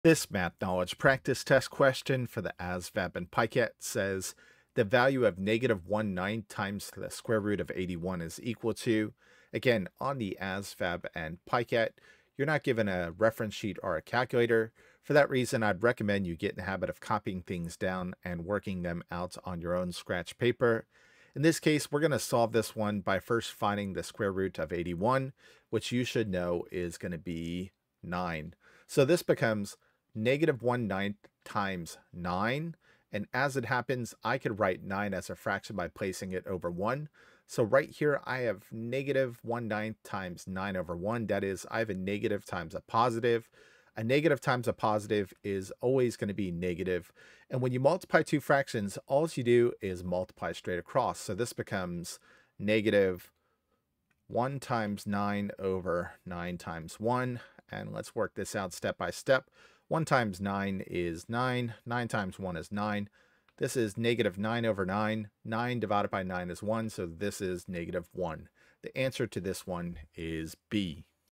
ASVAB/PiCAT Math Knowledge Practice Test Question: Square Roots and Perfect Squares
In this video, Grammar Hero works out a practice test question that requires you to identify a perfect square and its square root. This question should closely mirror what you should expect to see in the Mathematics Knowledge (MK) subtest of the Armed Services Vocational Aptitude Battery (ASVAB) as well as the Pre-screening internet-delivered Computer Adaptive Test (PiCAT). ​​​​​​grammarhero ​​ASVAB​​​​​